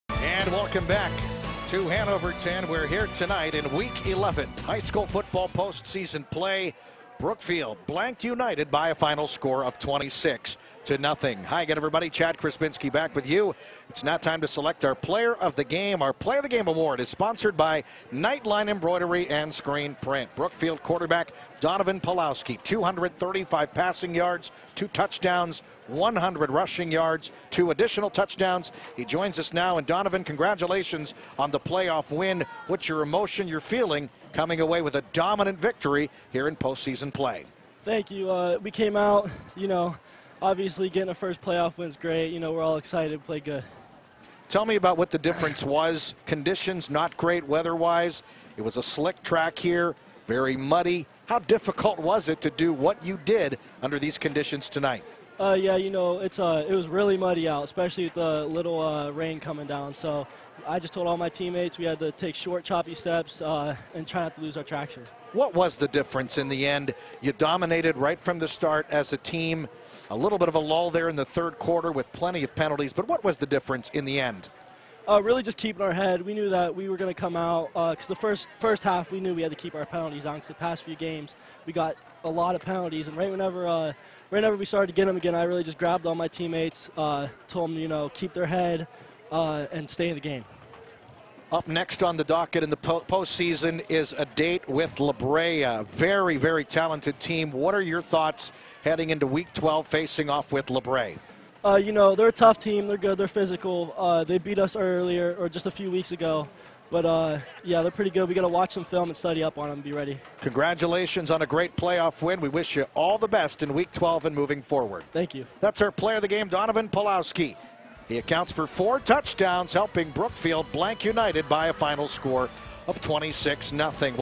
Player of the Game: Brookfield's Donovan Palowski
Brookfield Senior Quarterback Donovan Palowski was named Player of the Game for his efforts in the Warriors' 26-0 win over United Saturday night.